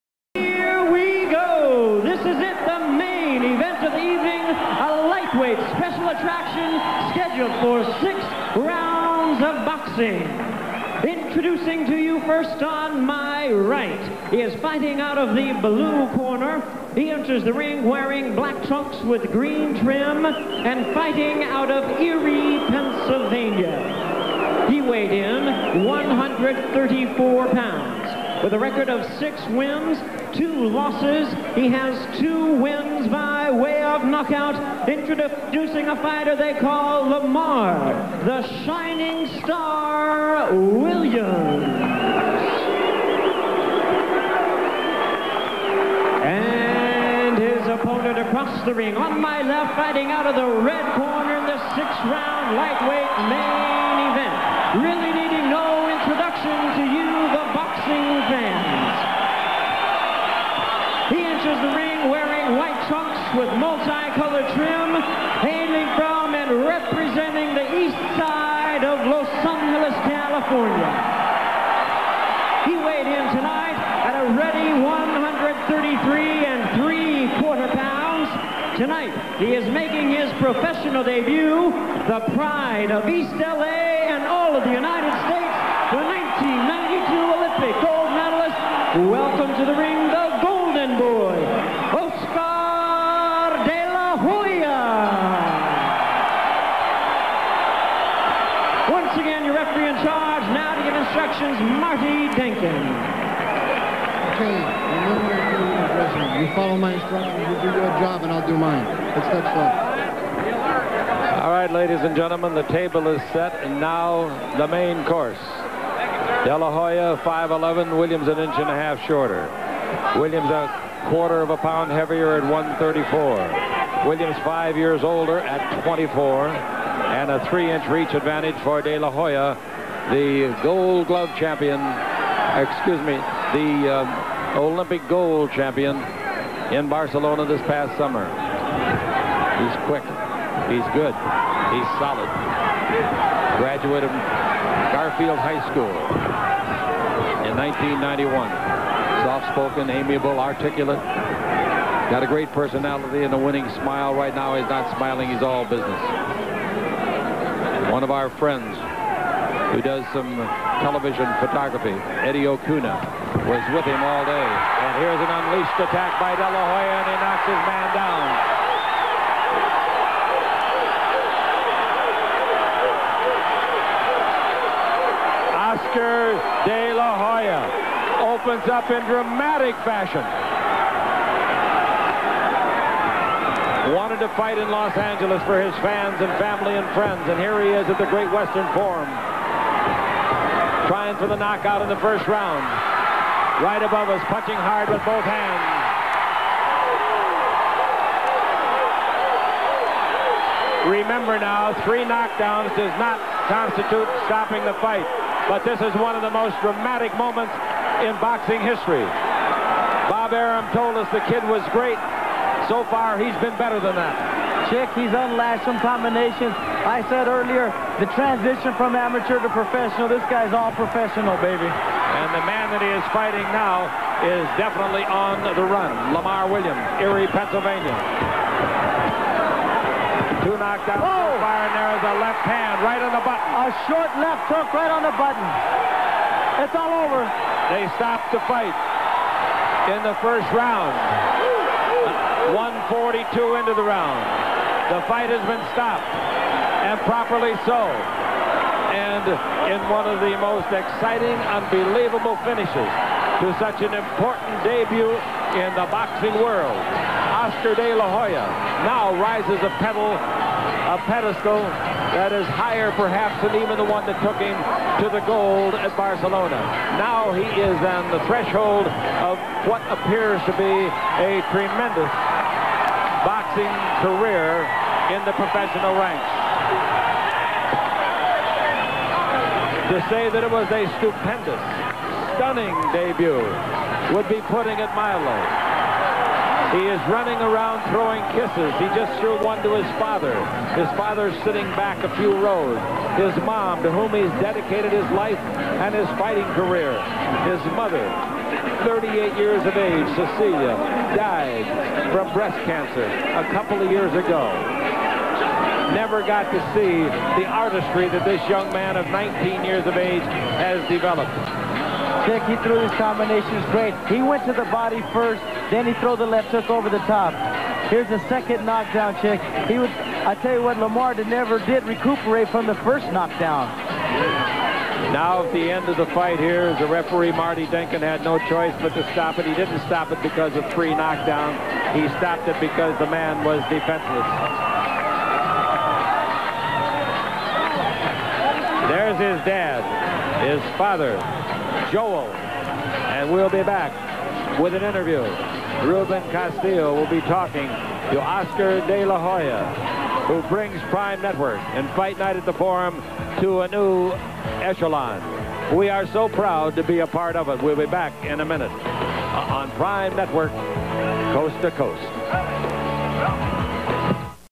1992.11.23 スーパーフェザー級 6回戦 オスカー・デ・ラ・ホーヤvsラマー・ウィリアムズ
『スーパーフェザー級 6回戦』

■ 1992年11月23日（月）会場：米国

オスカー・デ・ラ・ホーヤ（米国）

                    vs

ラマー・ウィリアムズ（米国）